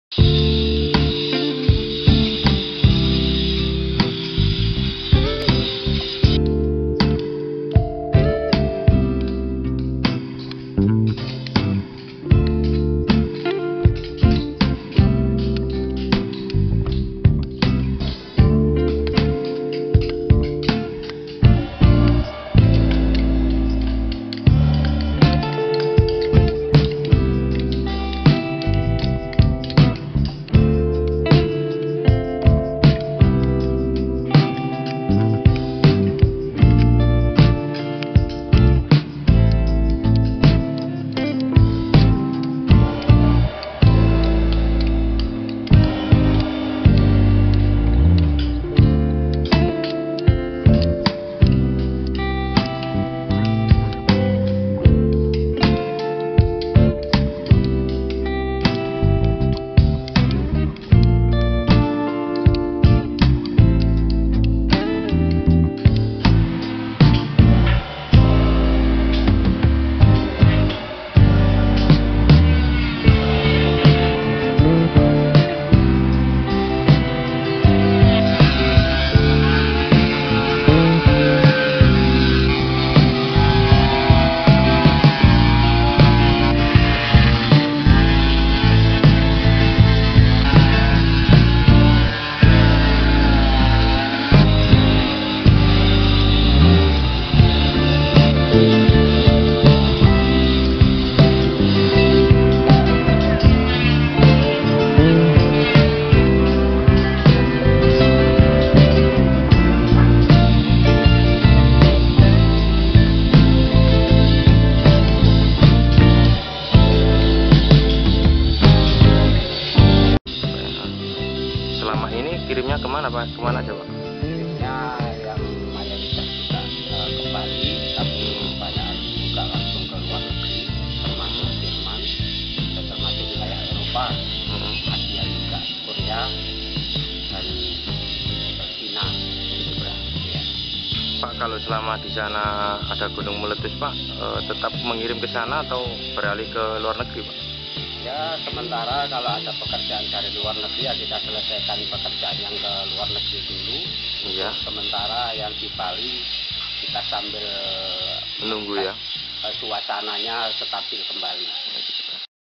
Mount Agung eruption
Volcanic Activity MOUNTAIN GREAT EXPERIENCE IN BALI THAT CONTINUES IMPROVEMENT / MAKING IMAGE MAKERS IN MOJOKERTO / EAST JAVA / SWITCH MARKET AND TYPE OF STATUE he made the craftsmen // RELY IF PREVIOUS FOREIGN TOURISTS IN BALI / WITH ANY SUCH ACTIVITIES Volcanology / The Craftsman SHOULD SWITCH MARKET OVERSEAS//

ERUPTION MOUNTAIN SUPREME IMPACT / IMPACT ON SOCIETY NOT ONLY IN BALI // BUT ONLY THE IMPACT OF SUCH been felt the craftsmen STATUE IN THE Trowulan / DISTRICT MOJOKERTO // SINCE THE OCCURRENCE OF MOUNTAIN GENERAL SYMPTOMS Volcanology / The Craftsman IMAGE MUST CEASE ITS BUSINESS IN BALI //

SUPPORTED BUSINESS SUMMARY INVESTMENT SUMMARY IN THE VILLAGE OF JATIPASAR / DISTRICT OF THIS TROWULAN / / SINCE THE MIDDLE 2017 PARTS / HOUSEHOLD BUSINESSES SHOULD HAVE BEEN EXPERIENCING THE LARGE ENOUGH IMPORTANT DECLINE // A LOT OF FOREIGN EXCHANGE WHICH CONSIDERING ITS DOWNTOWN BALI BECAUSE OF VULKANOLOGY ACTIVITIES //

DUE TO THE ACTIVITY OF THIS GUNUNG AGUNG / SALES OF THE PATENTS EXPERIENCED THE DECREASE OF THE OMZET // UNDERSTANDING THE DAILY DAYS / MAJAPAHIT TYPES OF SHEETS HINDU AND THIS BUDDHA / MANY OWNERS ABOUT FOREIGN CULTURERS OR / ORGANIZED PATENTS FOR SALE IN THE DENPASAR AREA AND BADUNG //

BUSINESS IMAGE TO REMAIN CURRENT CURRENT / Sutirto FINALLY TURNING SWITCH MARKET BY TYPE SCULPTURE sculpture // IF IN BALI HINDU NUANSA RELY ON IMAGE AND BUDDHISM / Sutirto turn it into STATUE OF ANY Moei / OR IMAGE PRIMITIVE are much in demand MARKETS EUROPE AND AMERICA //

KNOWLEDGE THE CHEAPFUL PRICE MOEI PRICE / BUT SIDE CURRENT LICENSE IS NOT AT THE CURRENT BALI // DURING THE NORMAL MOUNTAIN ACTIVITIES NOTE / THE PERMITS SHALL BE CONTINUE TO THE EUROPEAN AND AMERICAN MARKETS WITH STYLE STYLES // LEADERS PERSONAL LEARNING VULKANIC ACTIVITIES GUNUNG AGUNG KEMBALI NORMAL / AND CAN BUSINESS AS USUALLY //